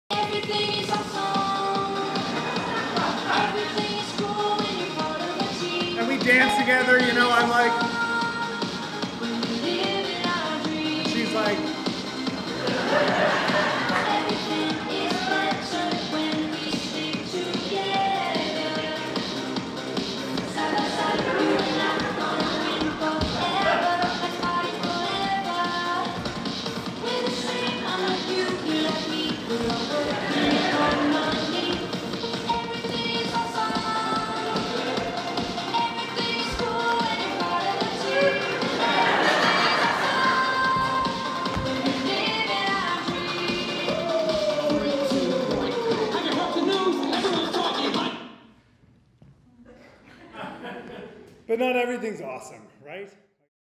Daughter | Sydney Festival 2019
A laugh-out-loud, yet appalling and riveting, confrontation of toxic masculinity.